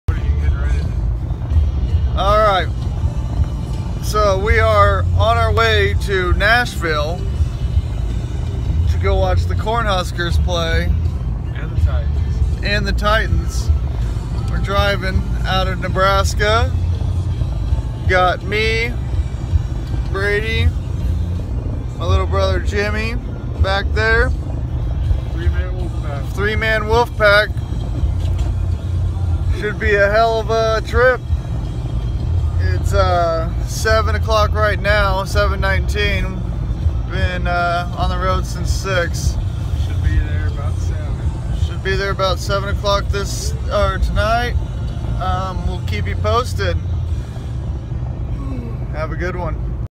3 man wolf pack road trip. The start!
I will not be streaming for the next couple days due to us going to the Nebraska bowl game and the Titans game against Huston in Nashville. Go Big Red!!